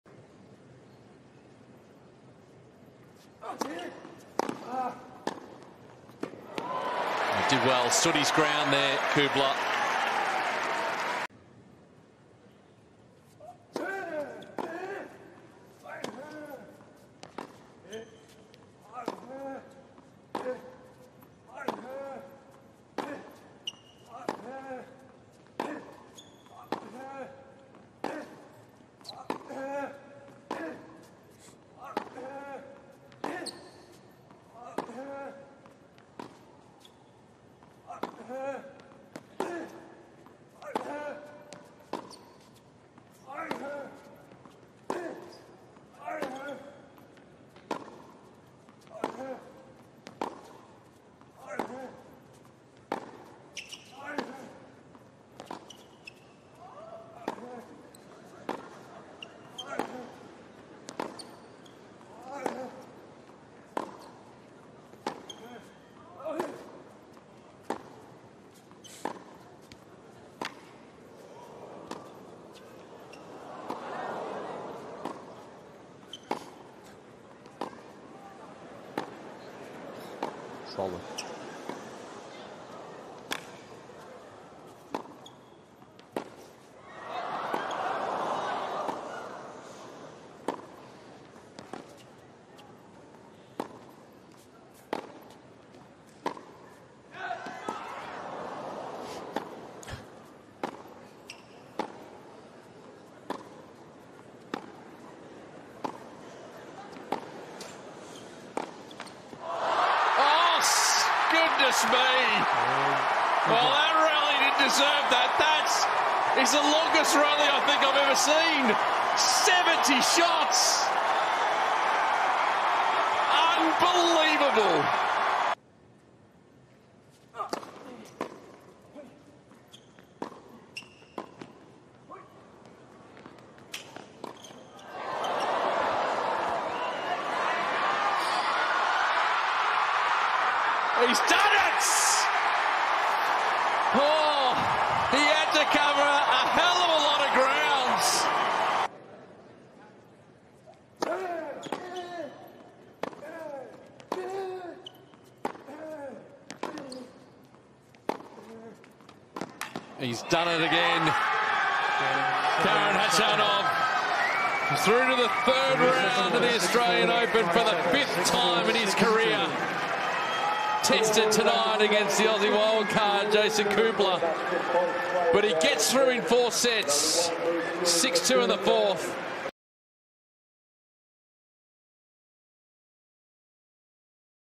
Karen Khachanov v Jason Kubler Highlights | Australian Open 2023 Second Round
Watch the highlights of Karen Khachanov v Jason Kubler in the second round of the Australian Open 2023.